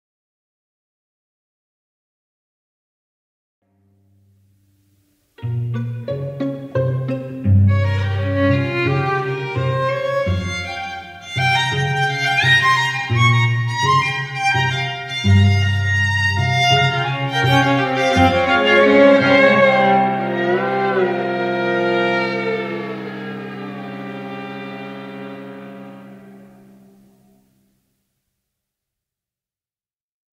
SoundBites: RNR1 Decca Tree + String Quartet
(3x) sE / Rupert Neve RNR1 Active Ribbon Microphones in a Decca Tree configuration, with a pair of sE4400a condensers in a wide AB configuration for ambience. Recorded through Merging Technologies Horus mic preamps, with minimal EQ.